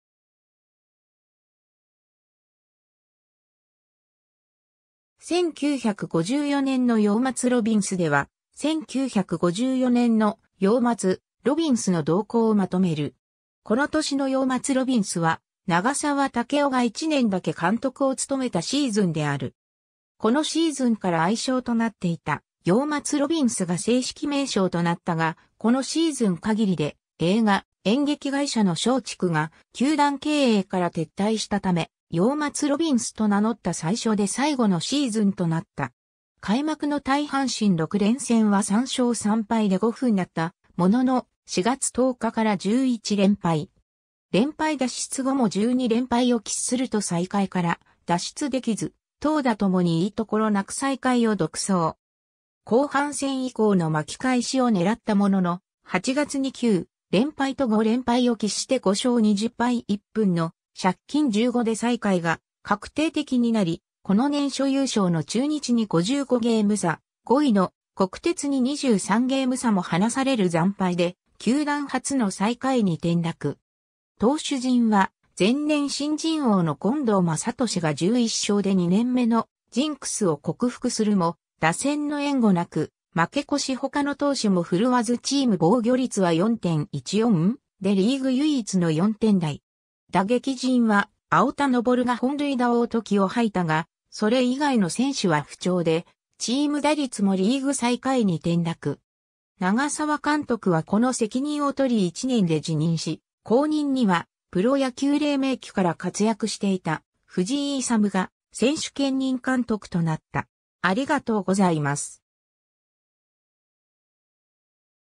1954年の洋松ロビンス
1954年の洋松ロビンス（1954ねんのようしょうロビンス）では、1954年の洋松ロビンスの動向をまとめる。
 この年の洋松ロビンスは、永沢武夫が1年だけ監督を務めたシーズンである。
 このシーズンから愛称となっていた「洋松ロビンス」が正式名称となったが、このシーズン限りで映画・演劇会社の松竹が球団経営から撤退したため、「洋松ロビンス」と名乗った最初で最後のシーズンとなった。
開幕の対阪神6連戦は3勝3敗で五分だったものの、4月10日から11連敗。
連敗脱出後も12連敗を喫すると最下位から脱出できず、投打ともにいいところなく最下位を独走。
後半戦以降の巻き返しを狙ったものの、8月に9連敗と5連敗を喫して5勝20敗1分の借金15で最下位が確定的になり、この年初優勝の中日に55ゲーム差、5位の国鉄に23ゲーム差も離される惨敗で球団初の最下位に転落。
投手陣は前年新人王の権藤正利が11勝で2年目のジンクスを克服するも、打線の援護なく負け越しほかの投手も振るわずチーム防御率は4.14でリーグ唯一の4点台（4位の広島でも3.81を記録）。
打撃陣は青田昇が本塁打王と気を吐いたが、それ以外の選手は不調でチーム打率もリーグ最下位に転落。
永沢監督はこの責任を取り1年で辞任し、後任にはプロ野球黎明期から活躍していた藤井勇が選手兼任監督となった。